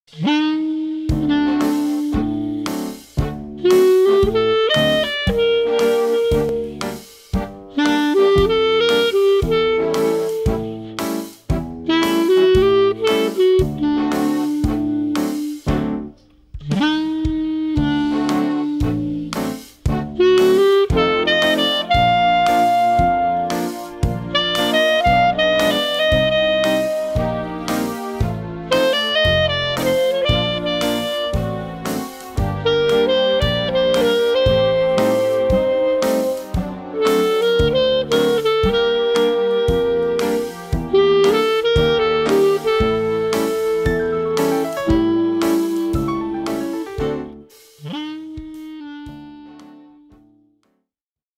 Nalbantov Clarinet Barrel Microphone
Handmade pickup microphone Nalbantov NCM series. Suitable for Clarinet, Sax, Bagpipe and Wooden flute /Kaval/